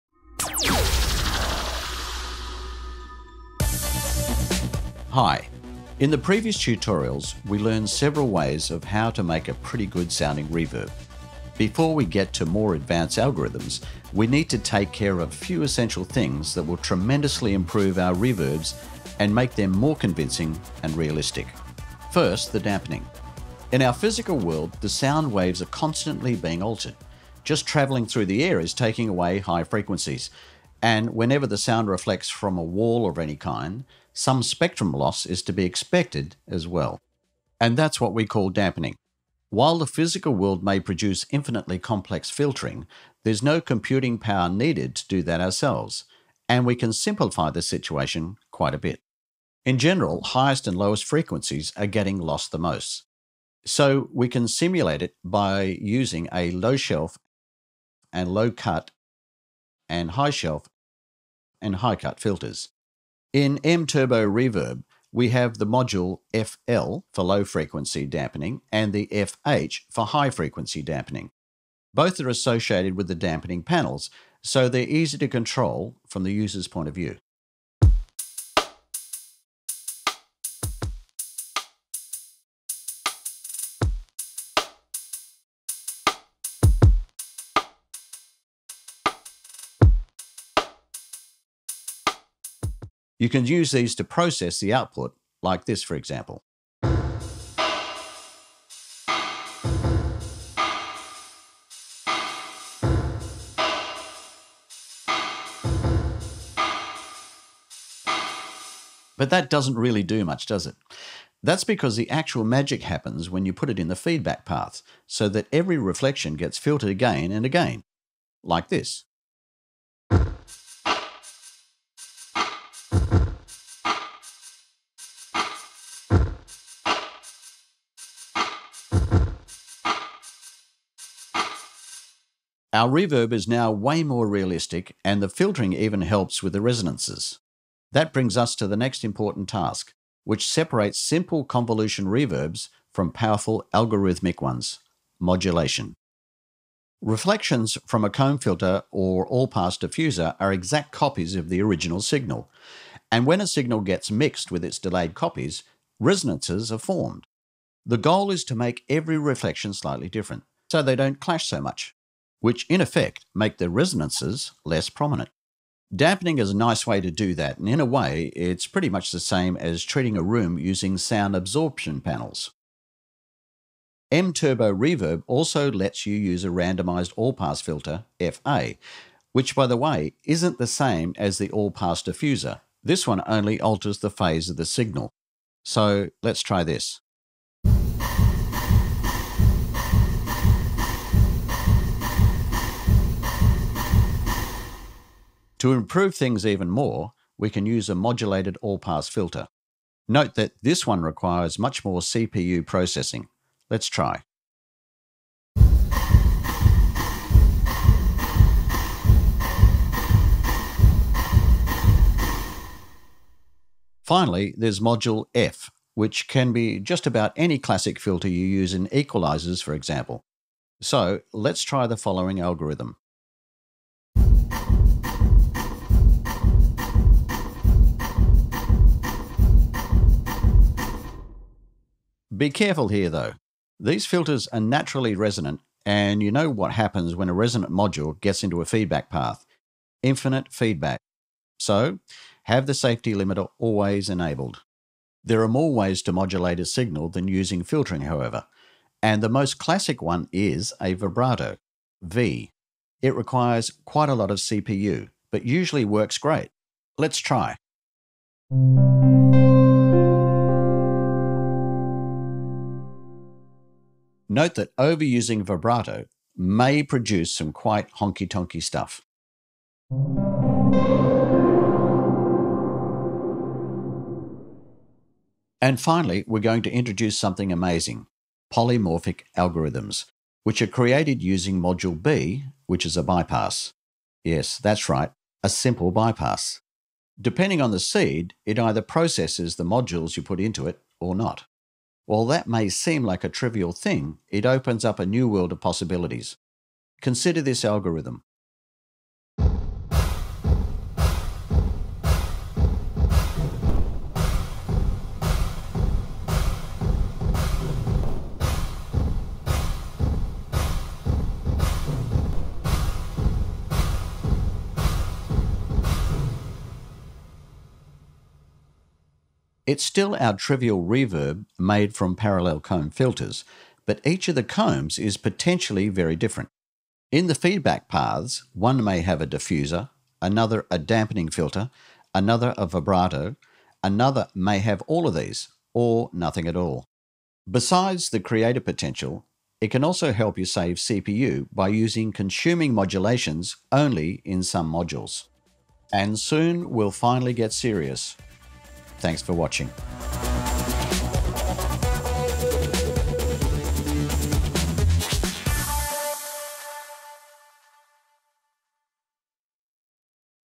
Reverb design using MTurboReverb #3 - Dampening, modulation and polymorphic algorithms
The 3rd video about reverb design shows essential concepts for making realistic reverbs - dampening and modulation. It also introduces the polymorphism, which makes the reverb design much easier.